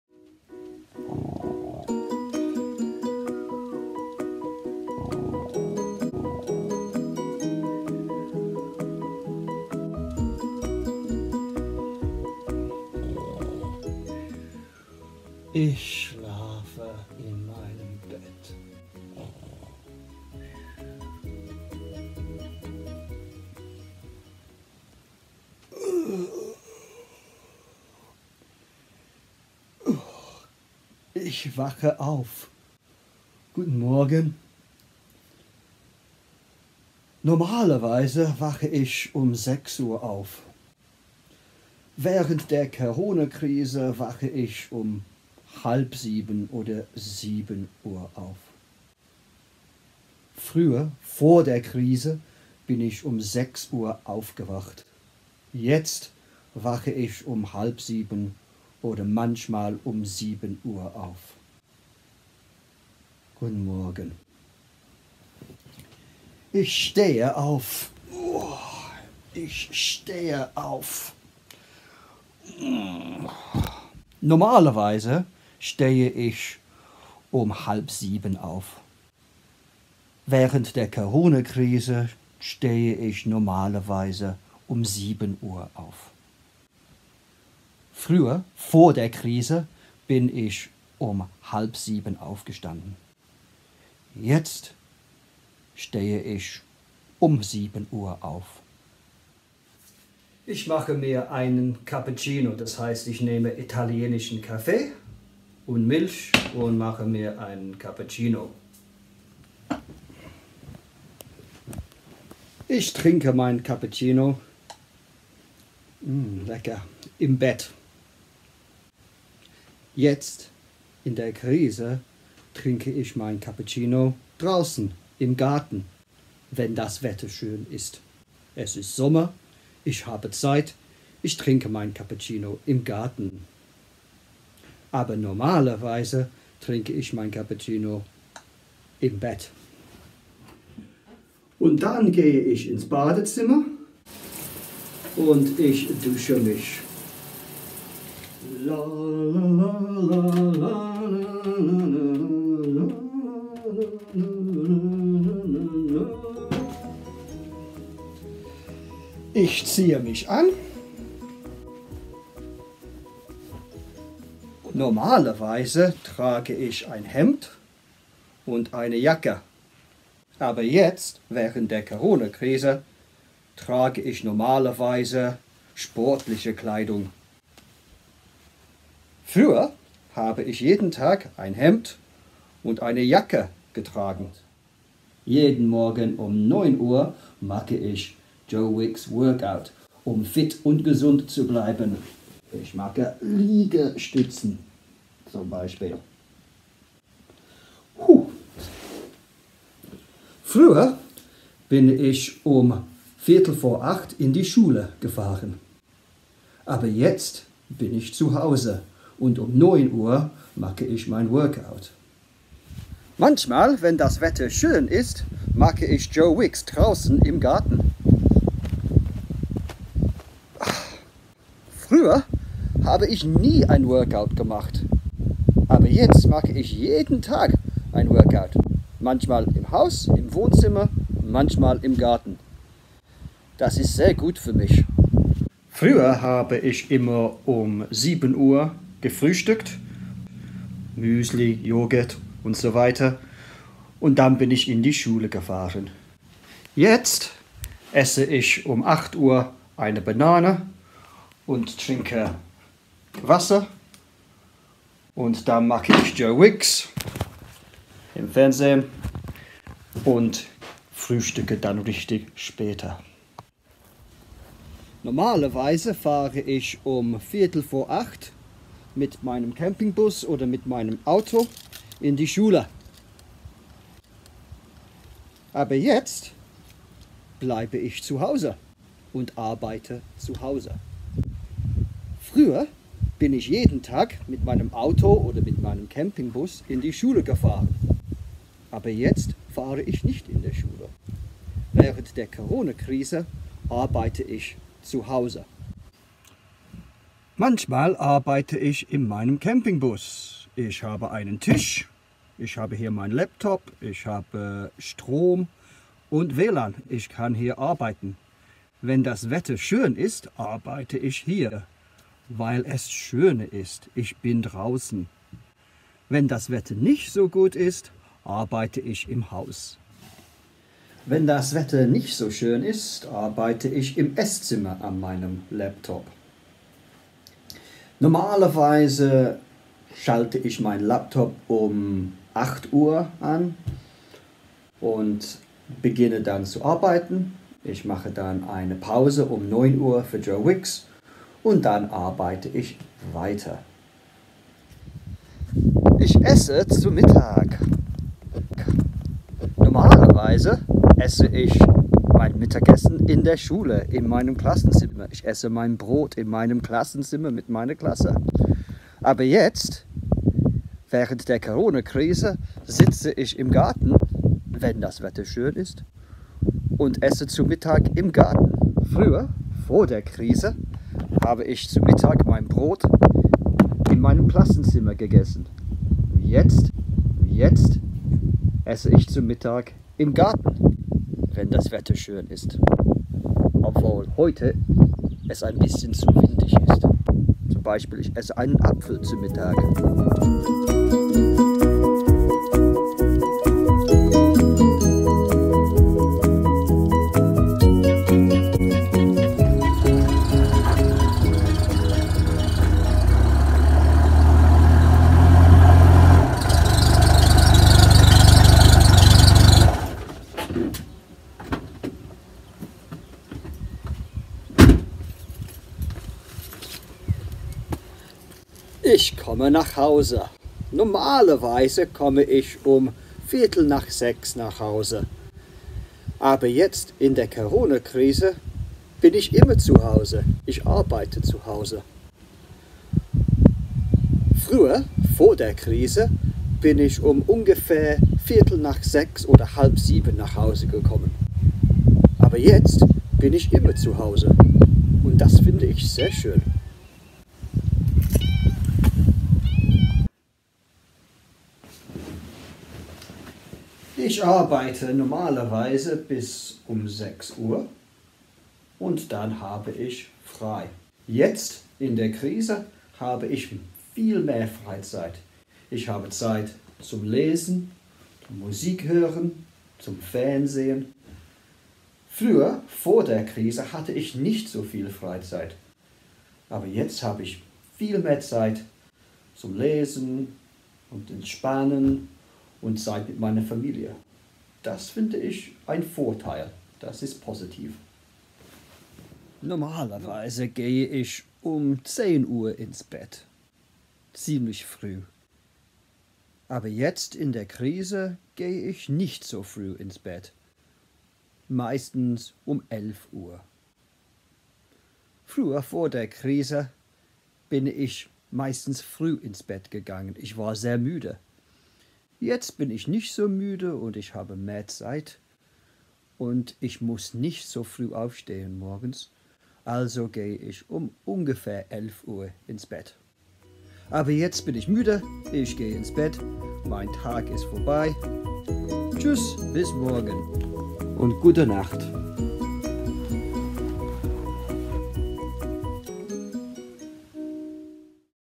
Der Alltag während der Corona-Krise (Present and Past Tenses)
Using present and past tense verbs to describe daily routine during the Coronavirus crisis and comparing it with what you used to do before the crisis.